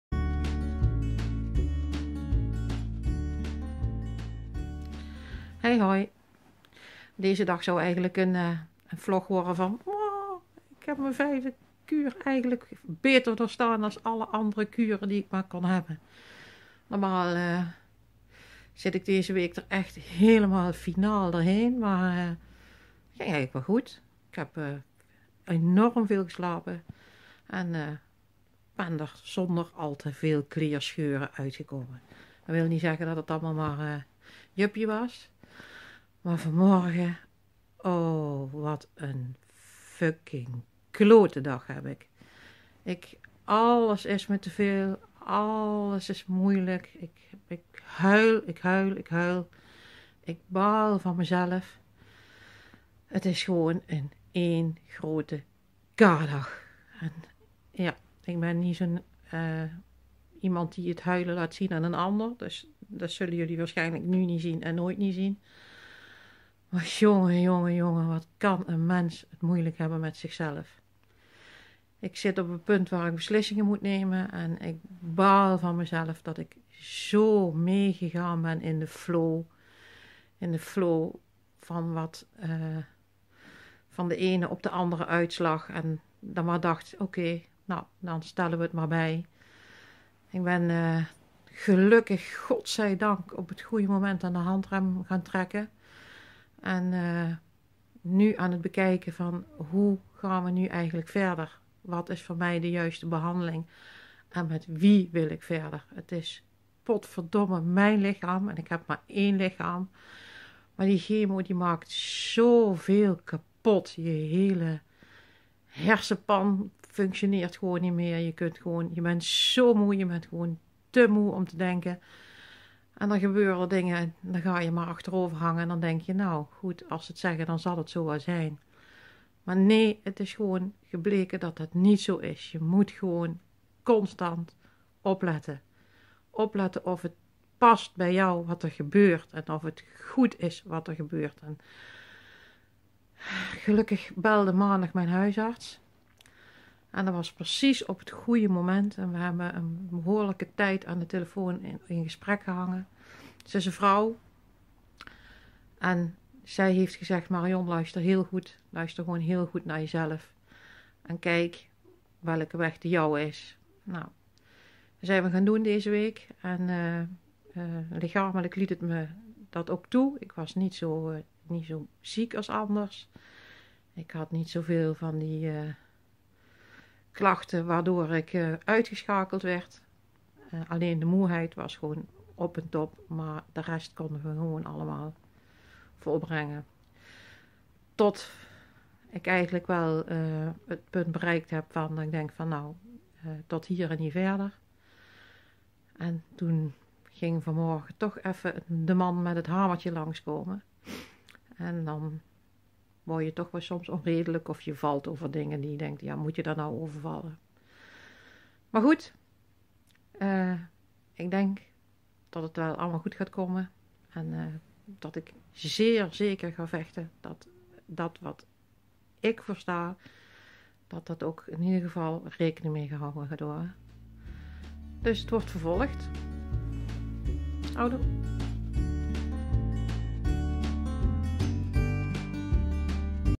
VLOG #04 Alles is teveel & hoe nu verder
Mijn wens is dat deze ellendige ziekte kanker, die mijn lichaam heeft uitgekozen om toe te slaan, iets positiefs brengt voor andere. Daarmee maak ik het ook een beetje tot een project, met een missie en dat past bij mij want alles wat ik doe in mijn leven doe ik met veel passie en positiviteit.